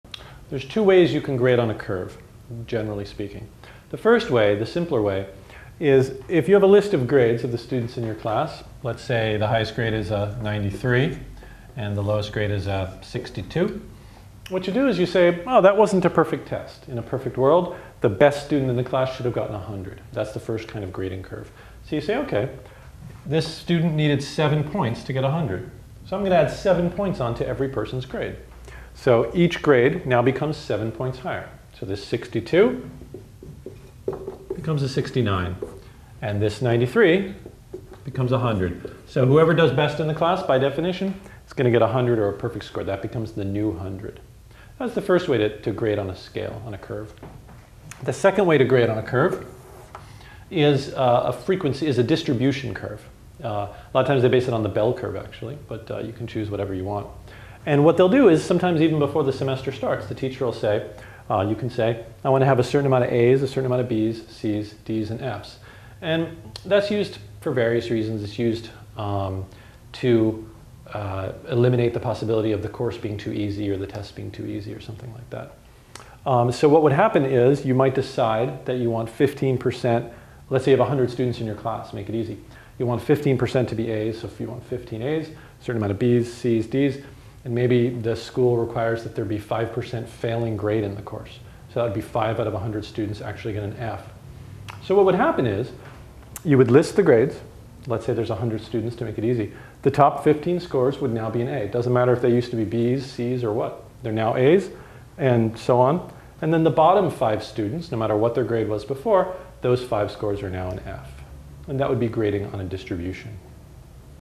Math Lessons & Study Tips : How to Grade on a Curve
Grading on a curve can be done by using a bell curve or a distribution curve. Find out how a score of 92 percent can become 100 percent in a grading curve with help from a tutor in this free video on math lessons and study tips.